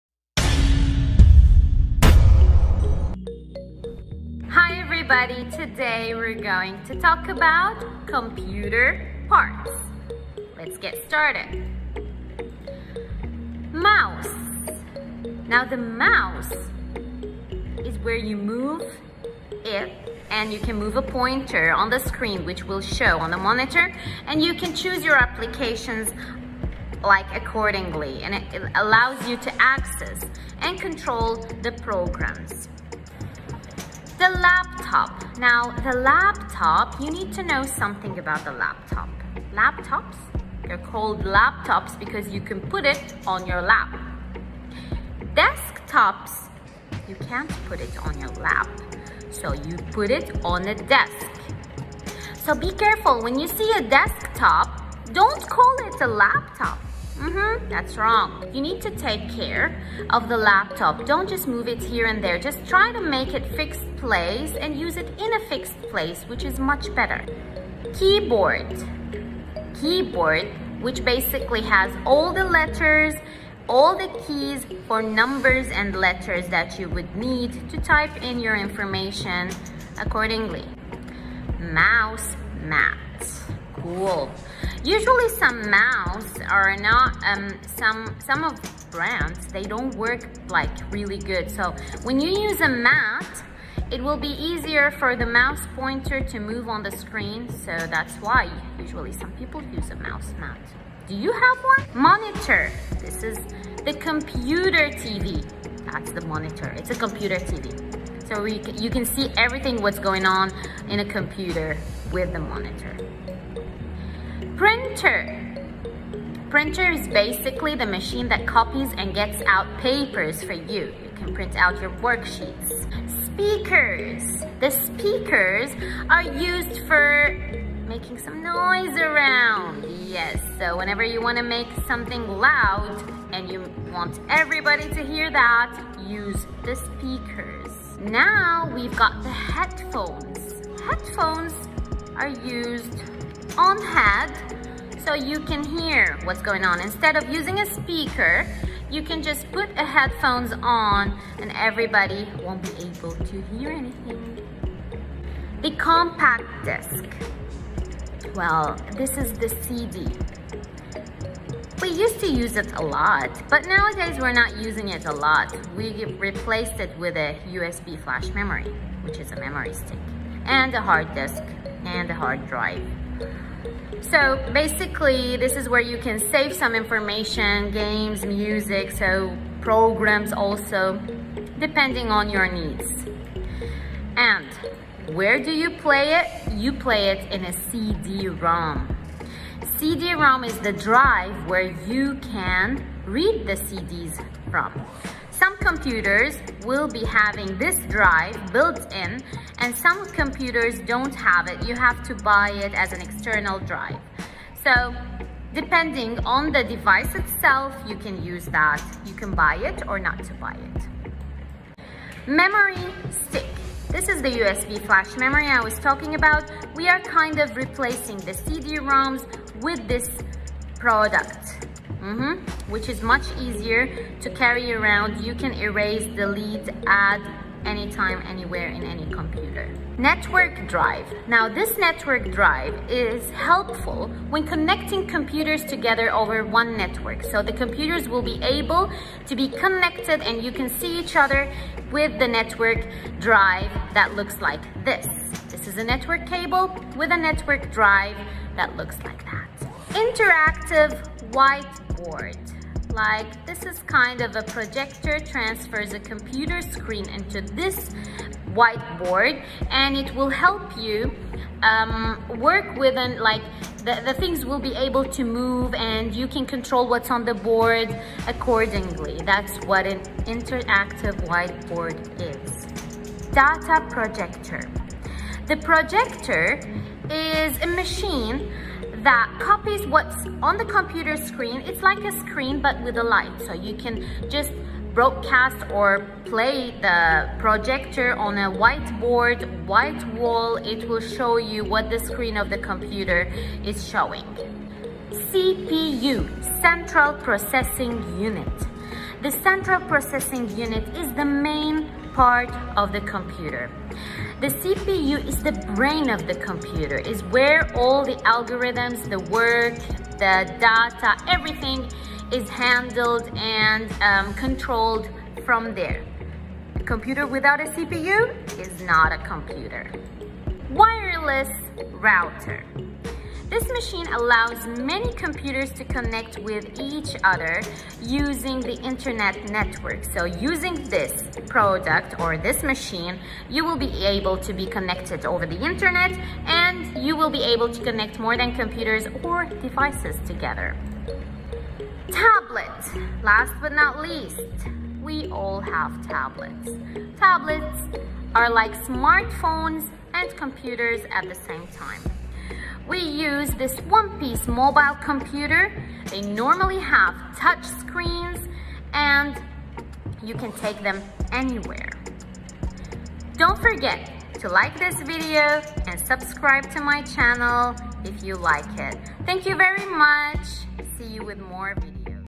Explaining Computer Parts
What are parts of the computer...
Explaining all the parts and discussing their features.